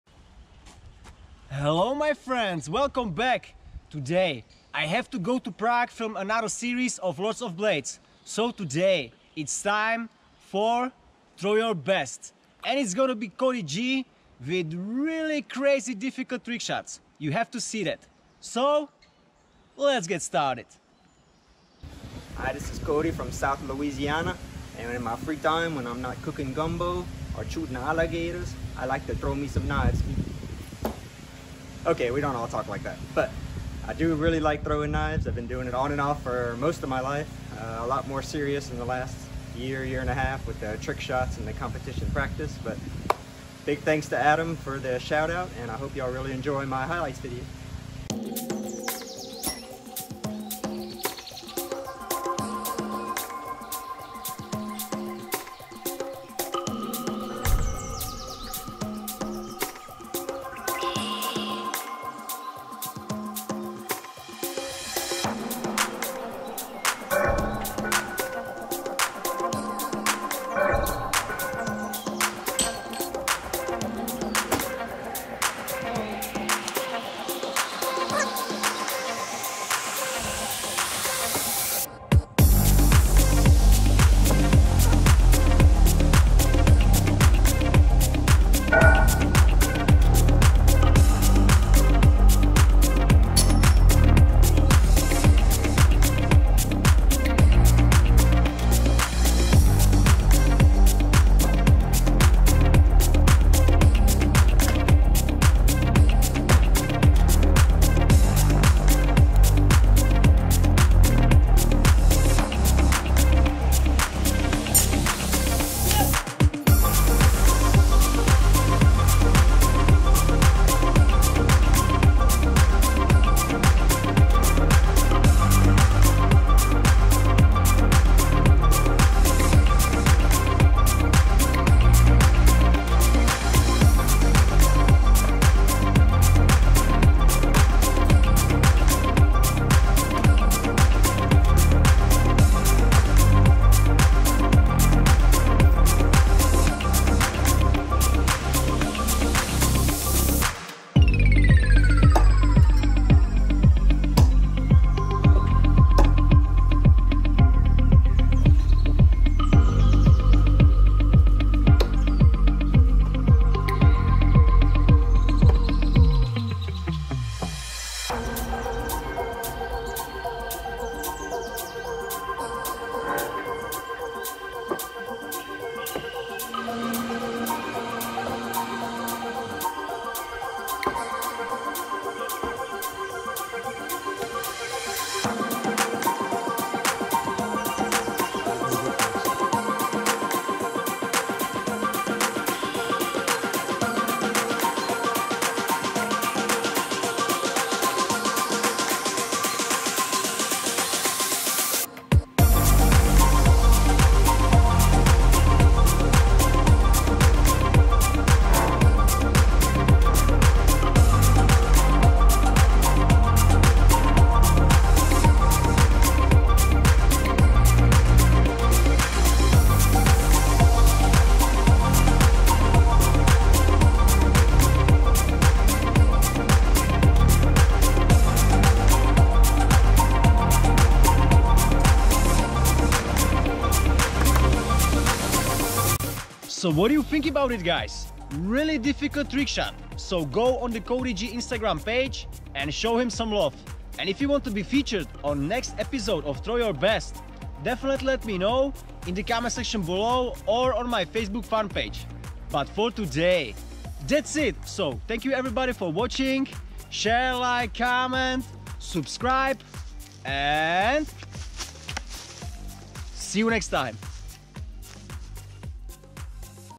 Blindfolded Knife Throwing Trick Shots (Cody G) Throw Your Best Ep.7
In New Episode of Throw your best we gonna see Really Great Trick-Shots by Cody G and some of them even Blindfolded...Thats Very Difficult! Enjoy

-if you missed last episode of throw your best, definitely Check it out:

Check out Cody g on Instagram: @codyg_la

Thank you everyone for Awesome support, I really appreciated :)